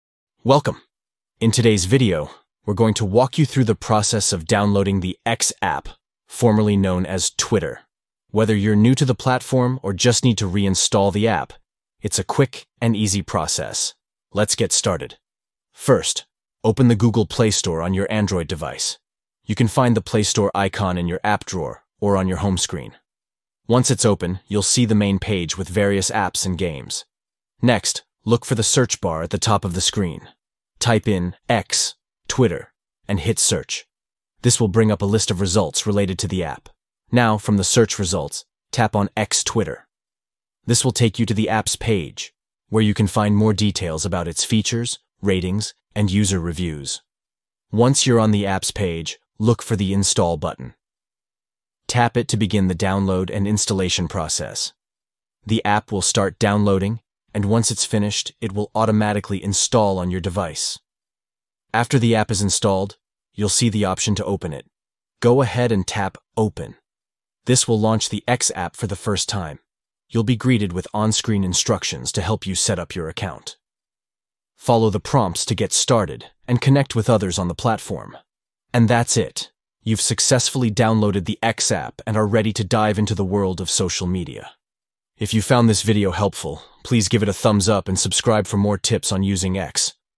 How to Download X (Twitter) App (Updated)
Learn how to download the X app with these simple steps! Whether you’re setting up a new account or reinstalling, this guide will help you get started quickly. Follow the steps below to begin.

Steps:
1. Open the Google Play Store on your Android device.
2. Search for "X (Twitter)" in the search bar.
3. Tap on "X (Twitter)" from the search results.
4. Tap the "Install" button to download and install the app.
5. Once the app is installed, you can open it and follow the on-screen instructions.

Chapters:
00:00 Intro
00:12 Soultion
01:05 Outro